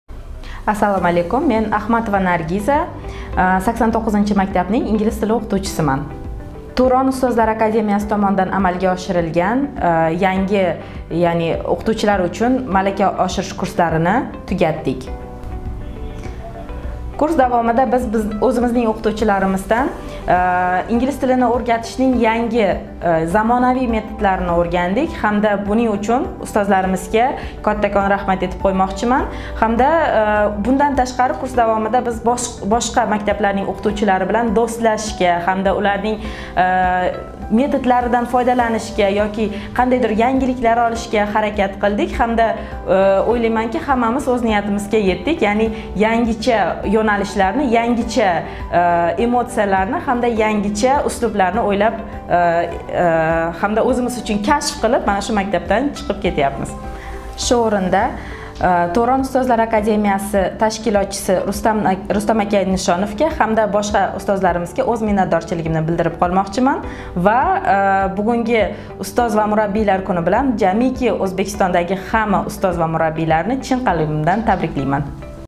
"TURON" ustozlar akademiyasi O'zbekiston tarixida birinchi... | Rustam Nishonov
💜"TURON" ustozlar akademiyasi O'zbekiston tarixida birinchi bo'lib maktab pedagoglarini malakasini oshirish huquqini qo'lga kiritdi.

🔥Endi malaka oshirish tizimida ham nodavlat ta'limning  samaradorlik, tezkorlik va sifat ko'rsatkichlari mavjud.

Mana shunday darslarda ishtirok etgan ustozlar fikri.

📲 𝗔𝗹𝗼𝗾𝗮: @RS_Markaz

Malaka oshirish va ustozlarni attestatsiyaga tayyorlash darslari haqida xabarlar ushbu guruhda to'liq yoritib boriladi: